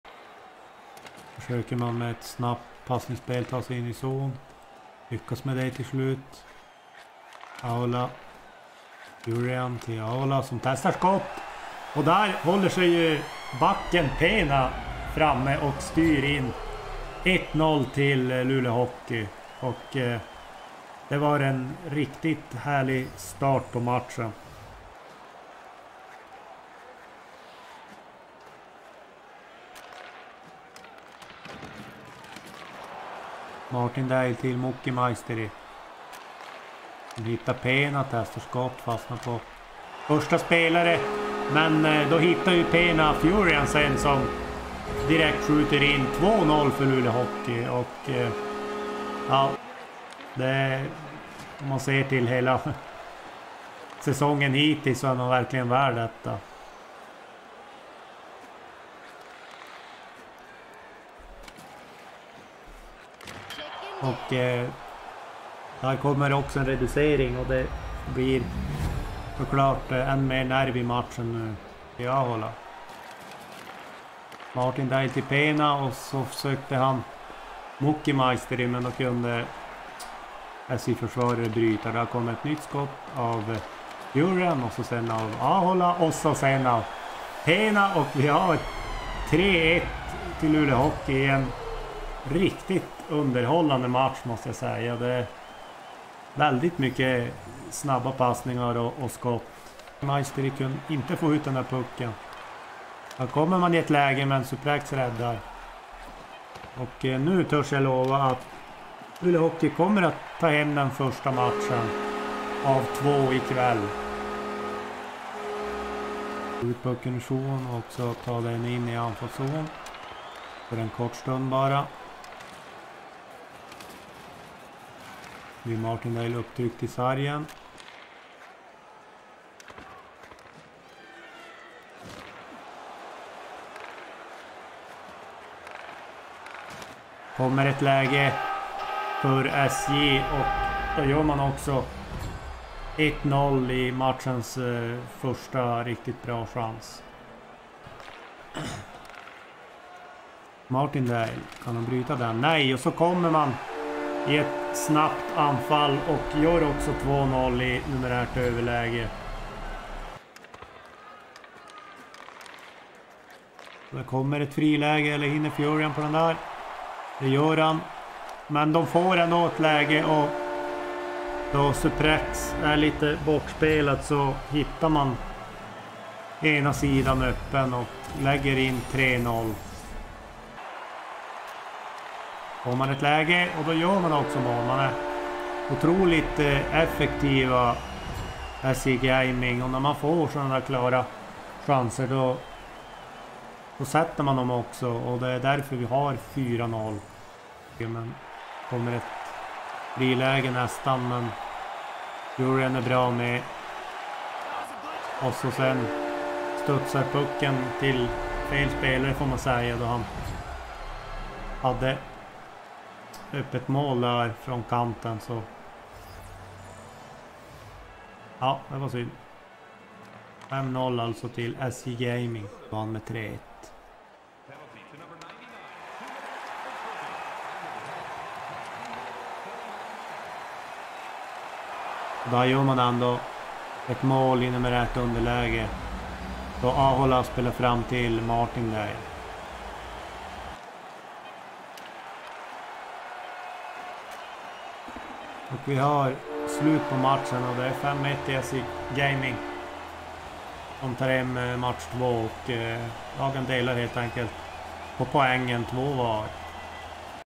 Highlights Luleå Hockey - SJ Gaming ECL Elite round 8
Luleå Hockey - SJ Gaming 3-1
SJ Gaming - Luleå Hockey 5-1

Editing: Andreas Joanson